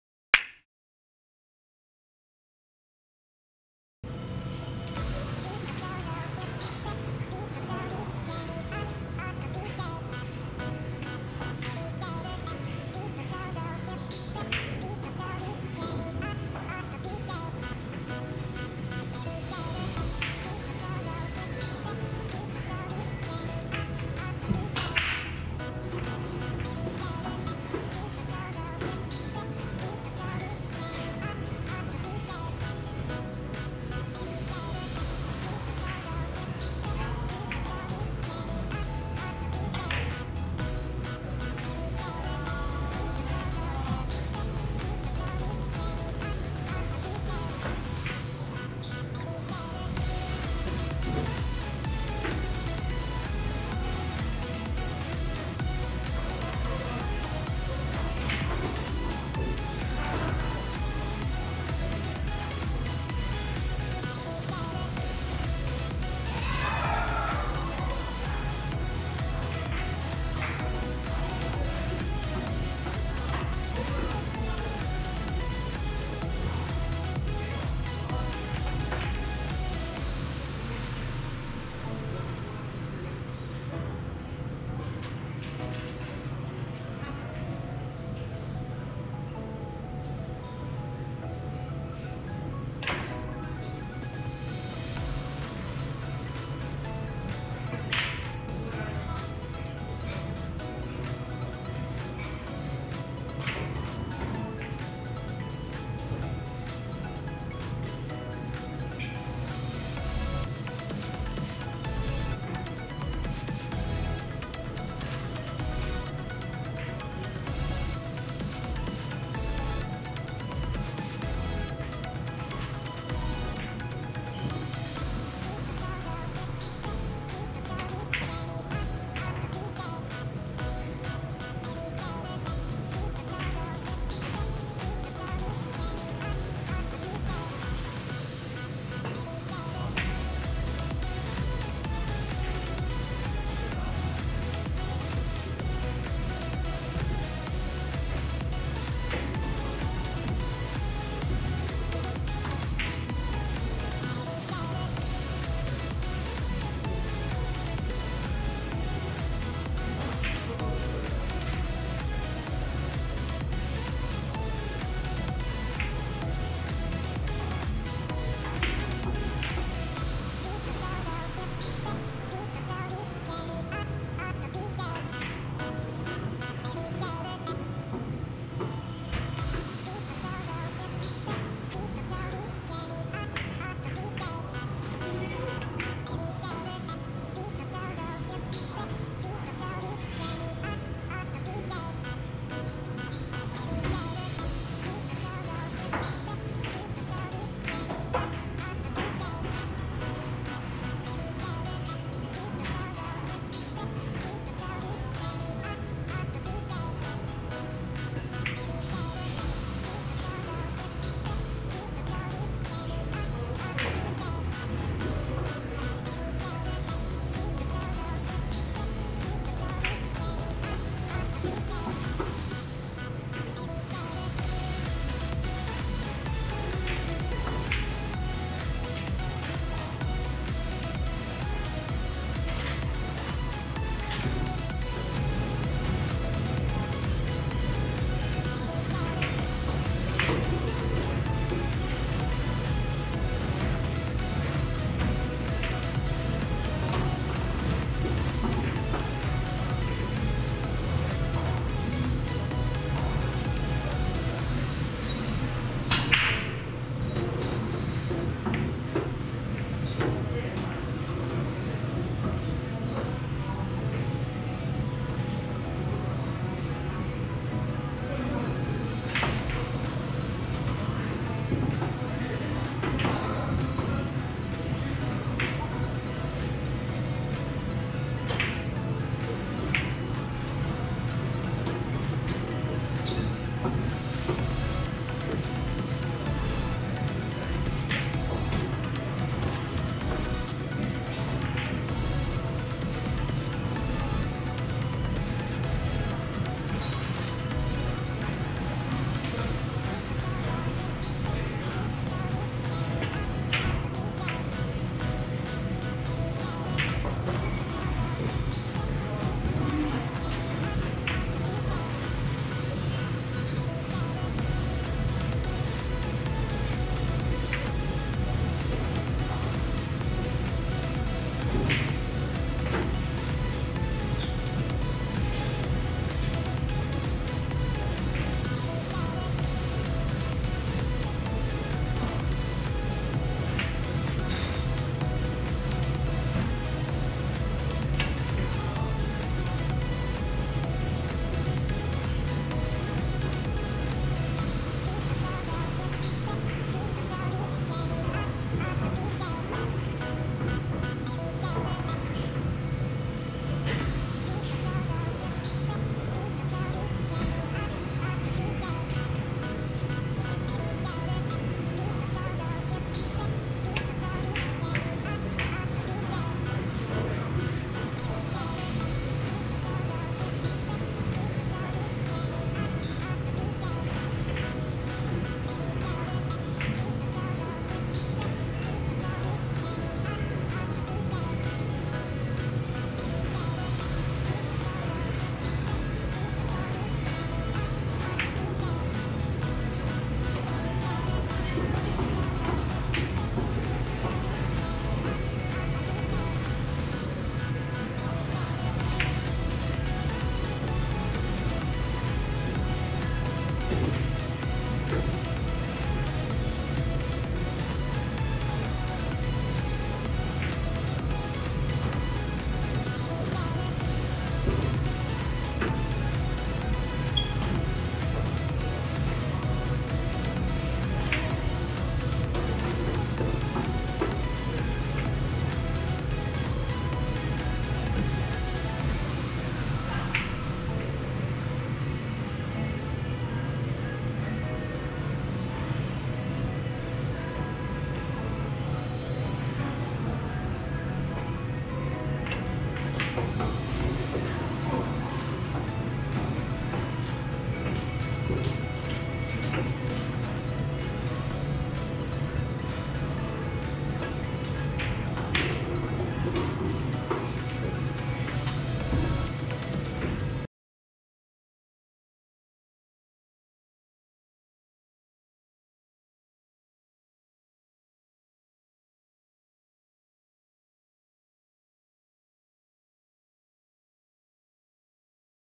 【沖縄ビリヤード】2022年11月 BC級しか勝たん@BAATA 外原VS石原【ONPC】| VLOG #318
2022年11月 BC級しか勝たんの動画。